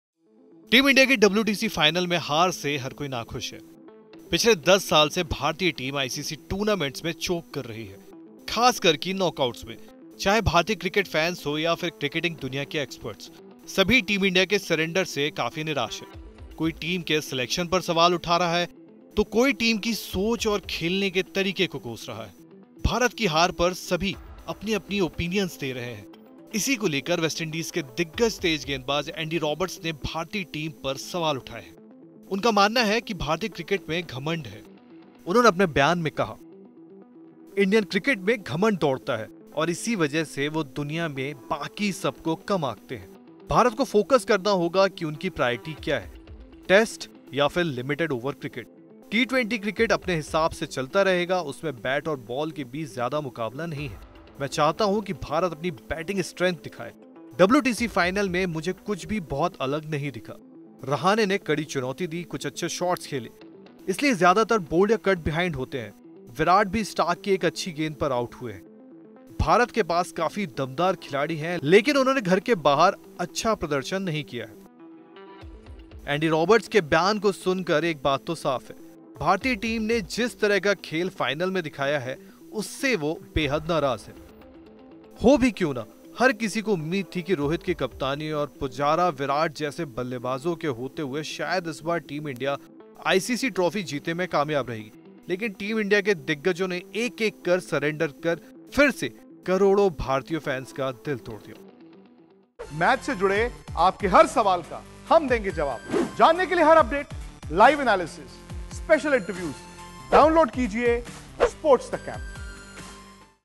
'Indian Cricket को घमंड ले डूबा', क्यों WI के Legendary Andy Roberts को देना पड़ा ऐसा बयान?
‘Arrogance has crept into Indian cricket’: Sir Andy Roberts slams Team India